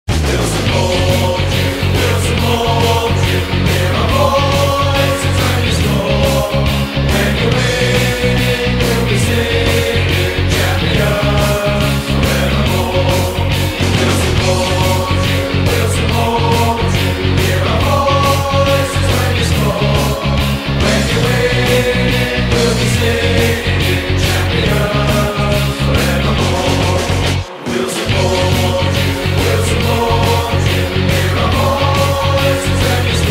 Fallaccio di Burgnich su Gaspare Umile (Inter-Napoli 21.03.1971)
Fallaccio di Burgnich sul più grande calciatore marsalese della storia: Gaspare Umile.